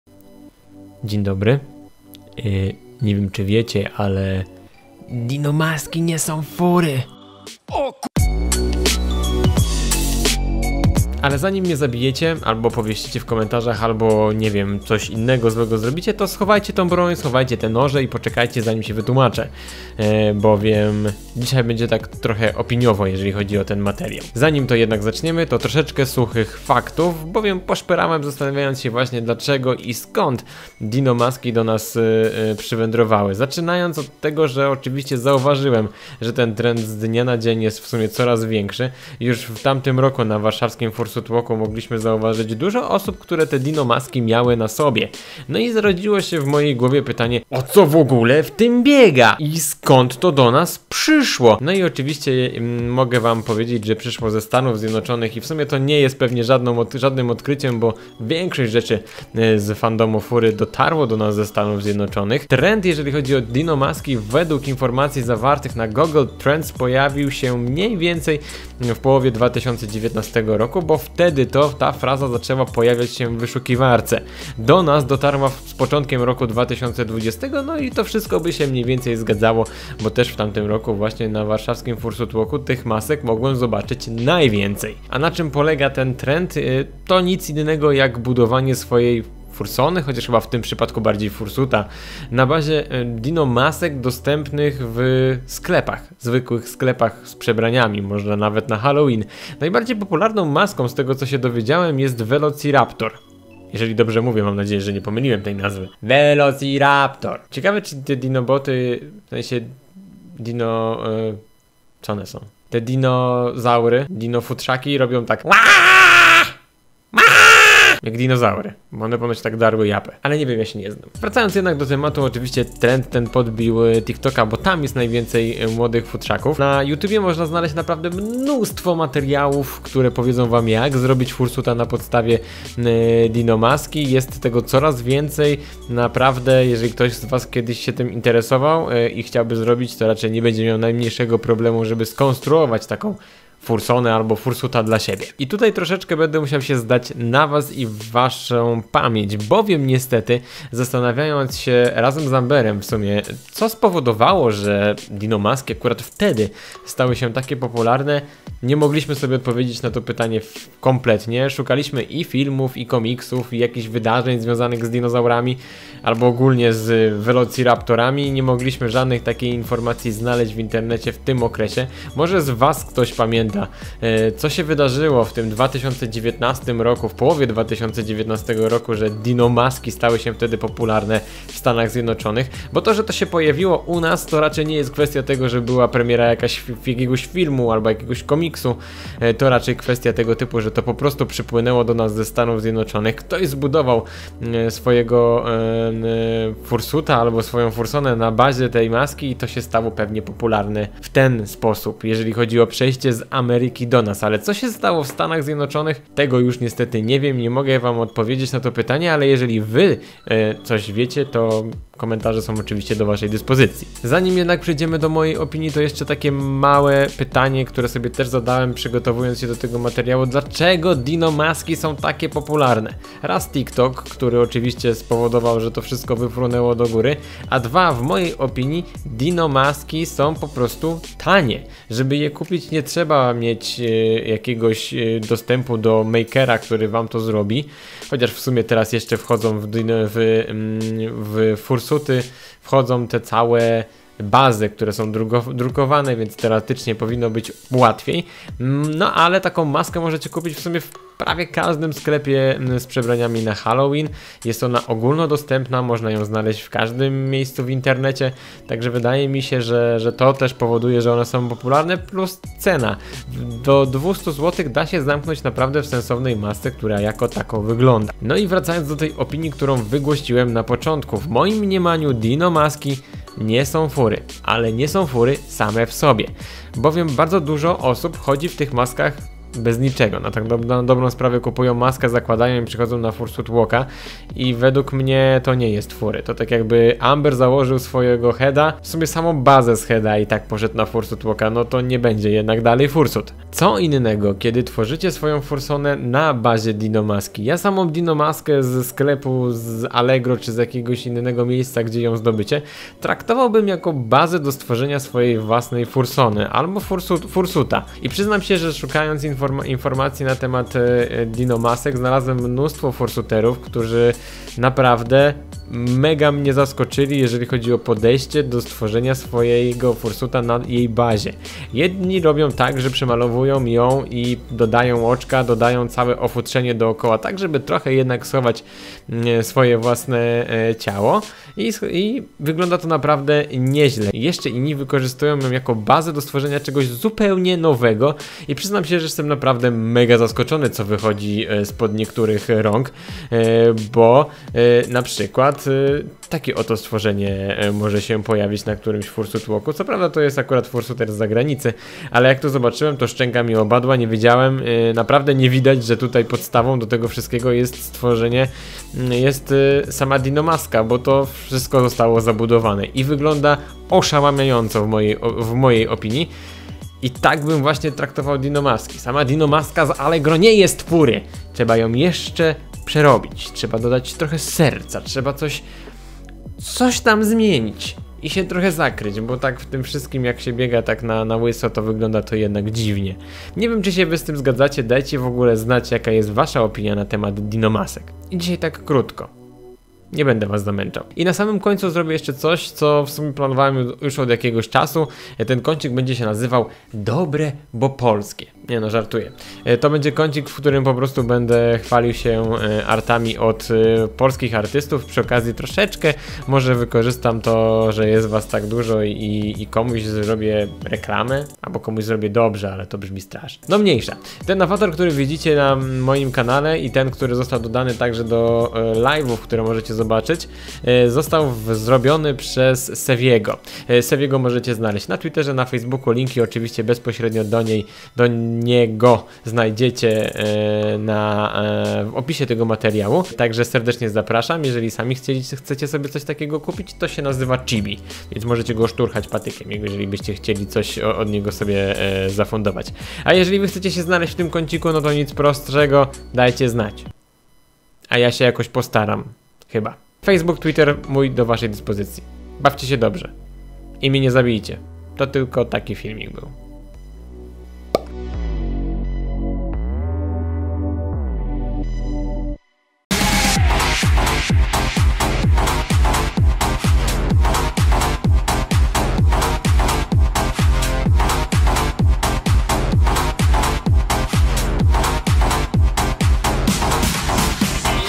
Dino Maski, o co w tym chodzi? | Fandom Furry
Coraz większą popularnością cieszą się Dino Maski. Jest ich coraz więcej, ale skąd się one wzięły? Na czym polega ten trend i przede wszystkim, czy dino maski są furry? Na te pytania spróbuje odpowiedzieć w tym materiale.

Kicek